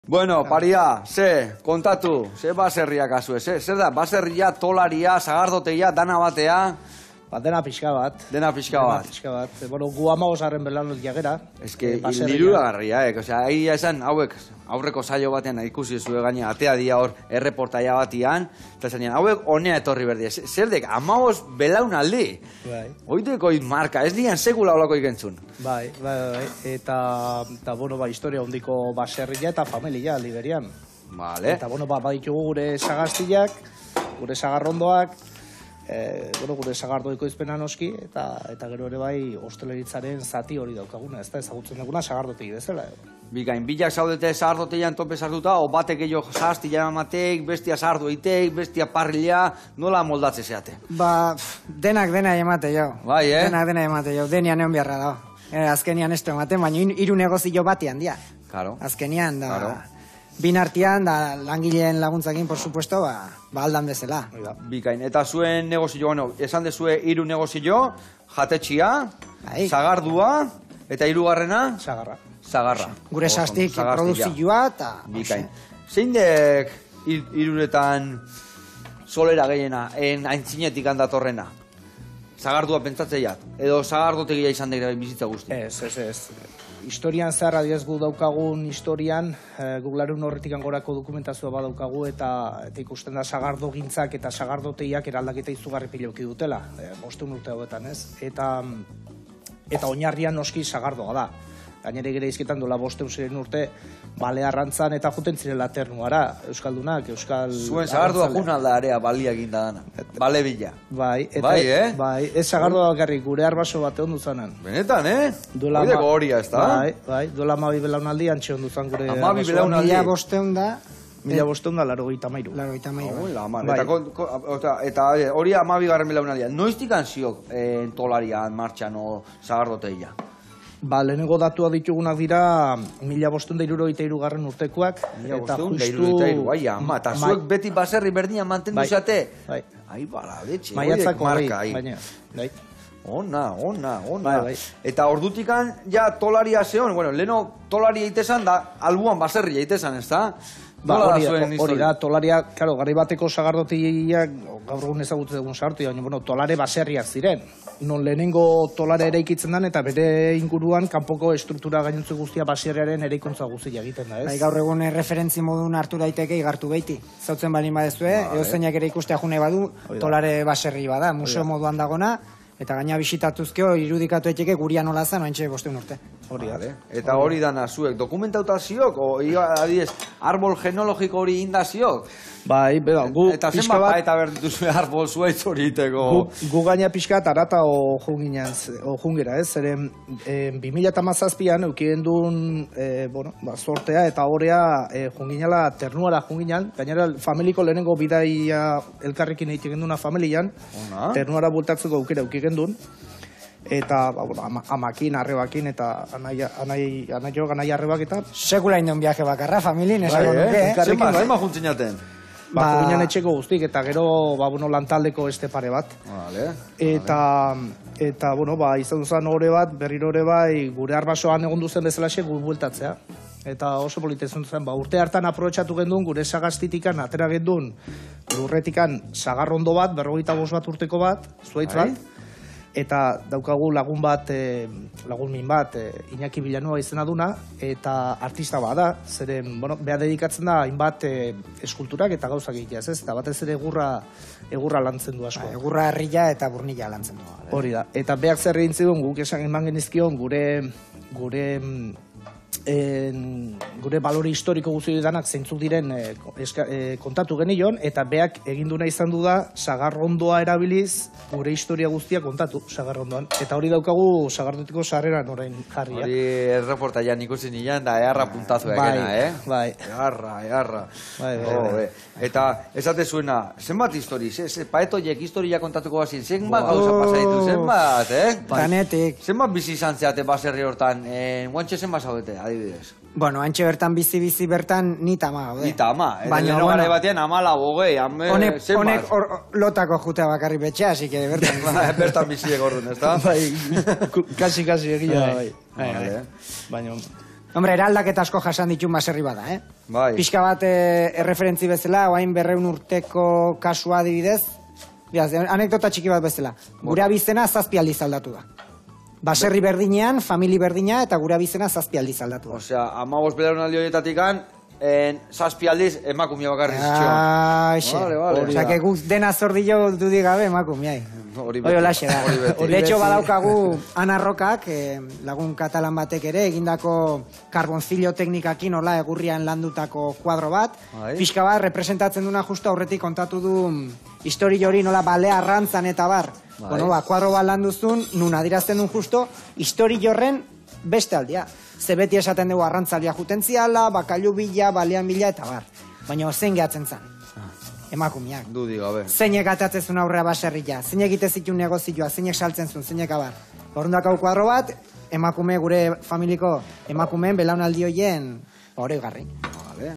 Hur eta Joseba Astarbe: ''Mendiola baserriko 15. belaunaldia gara''
Astarbe sagardotegiko bi anaiak izan ditugu gonbidatu eta bere baserri eta familiaren historia harrigarria kontatu digute. Euren sagardoa Ternuaraino iritsi da!